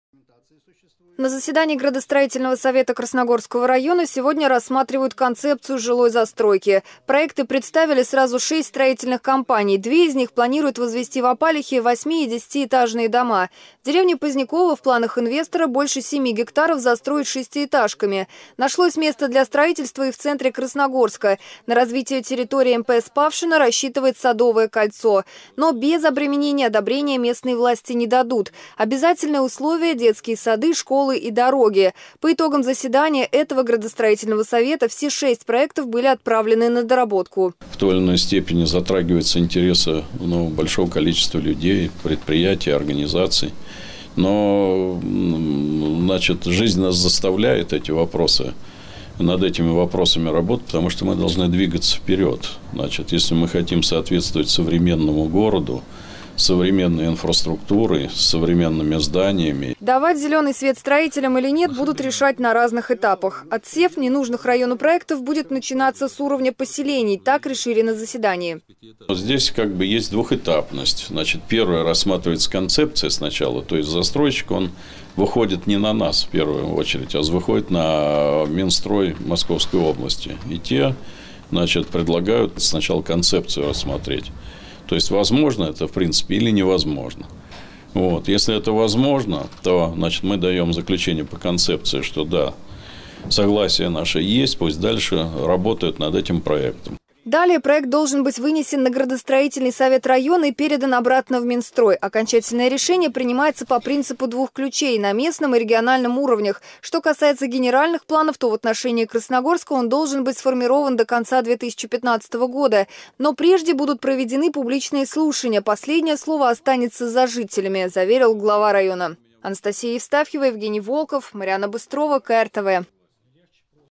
Градостроительный Совет Красногорского района
В Красногорске прошло первое заседание Градсовета в новом формате и обновленном составе. Коррективы в работу совещательного органа были внесены в связи с перераспределением полномочий и реализацией принципа «двух ключей», озвученного губернатором Московской области Андреем Воробьевым.  Градостроительный совет оценивает целесообразность предложенных планов строительства на территории района.